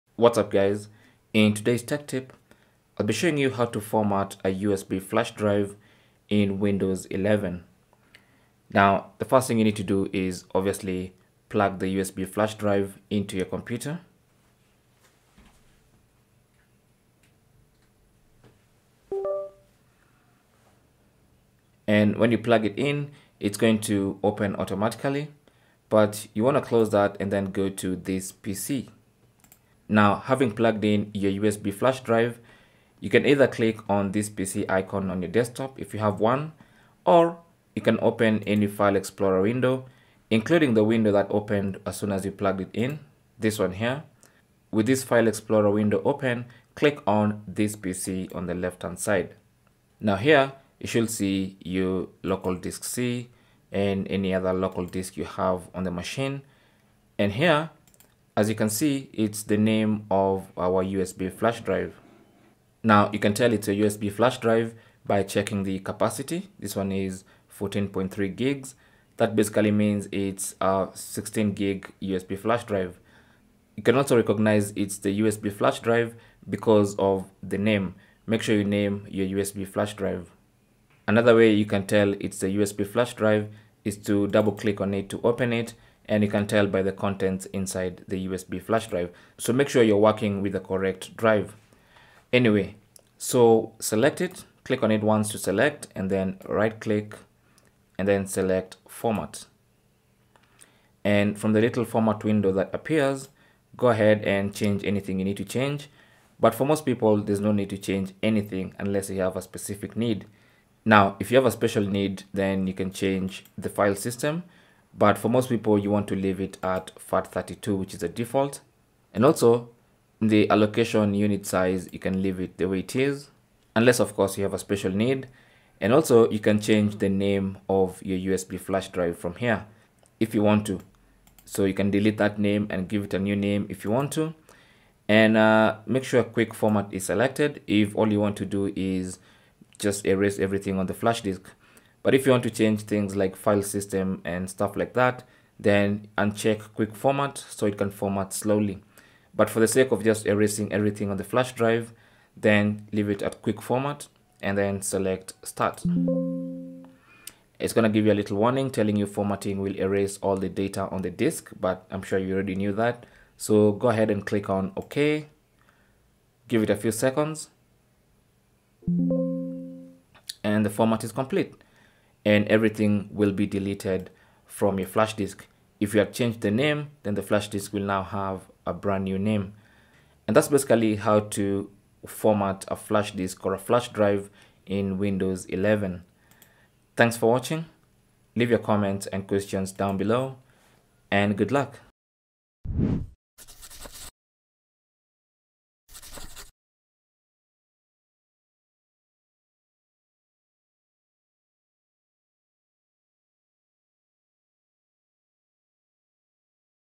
How To Format USB Flash Drive In Windows 11
Learn how to format a USB flash drive on Windows 11.
You might want to create space on your flash drive by formatting it. In Windows 11, you can easily format your flash drive. However, your data will be erased from the flash drive once you format it. We recommend backing up any important files on your flash disk before formatting.

Insert the flash drive into your PC or laptop
Back up any Important Files from the flash drive.
Right-click on the USB flash disk and select 'Format'.
From the Format pop up Window, select 'Quick Format'
Confirm you want to Format your flash drive
Once complete, eject the USB Flash Drive

Windows 11 Specifications
Edition: Windows 11 Pro
Version: 21H2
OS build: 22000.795

Cell Phone Tripod Adapter